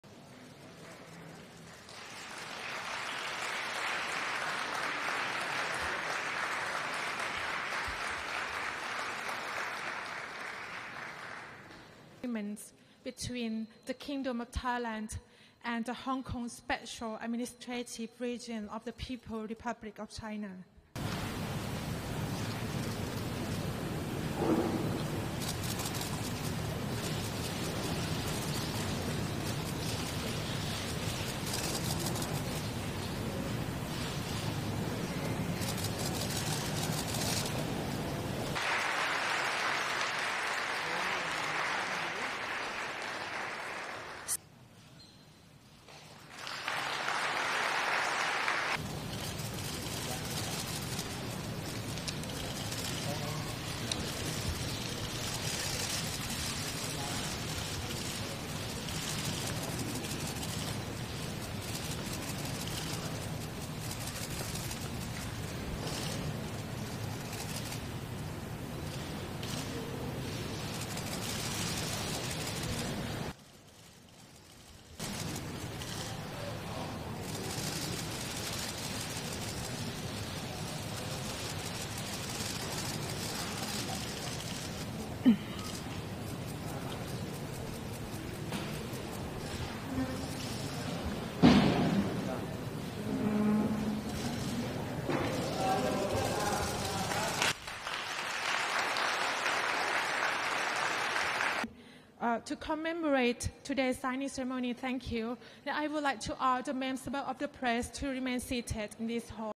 Lam seeks to strengthen ties with Thailand
(29 Nov 2019) Hong Kong Chief Executive Carrie Lam, Thailand's Deputy Prime Minister Somkid Jatusripitak and representatives from both regions signed six memorandum of understandings (MoUs) on Friday.
The MoUs aim to bolster trade ties and increase investment between Thailand and Hong Kong.
Earlier, Lam praised Thai lawmakers for promptly preparing the MoUs.